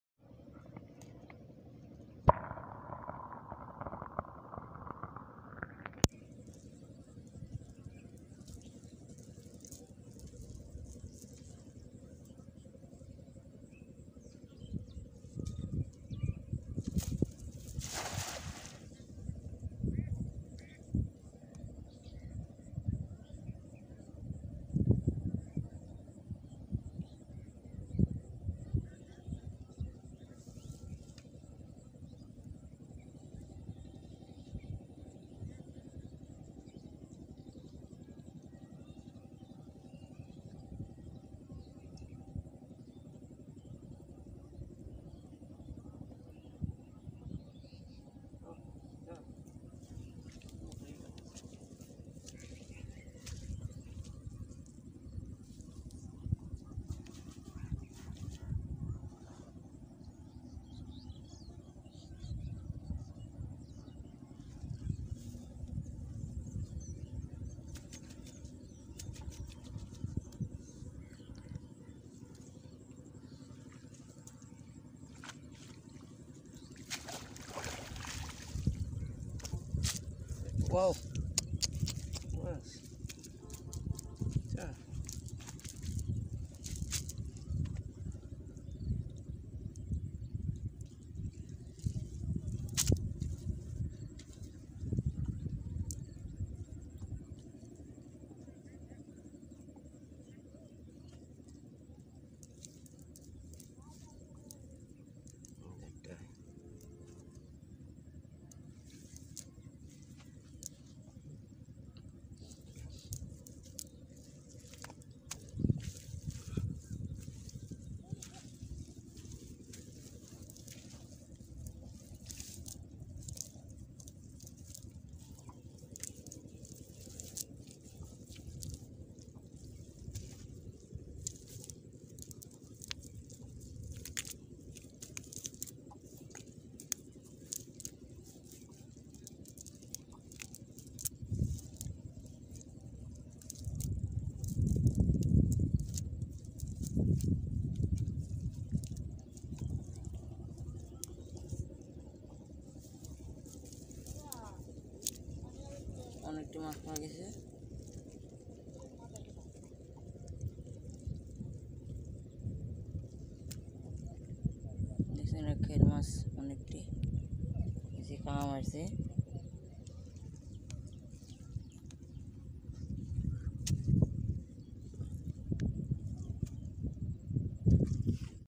Bast Net Fishing By Village Boys Very Amazing Fishing in River#fishing
ঝিনাইগাতি শেরপুর জেলা
নালিতাবাড়ী ময়মনসিংহ 'শেরপুর বনগাঁও খন্দকার পাড়া গ্রাম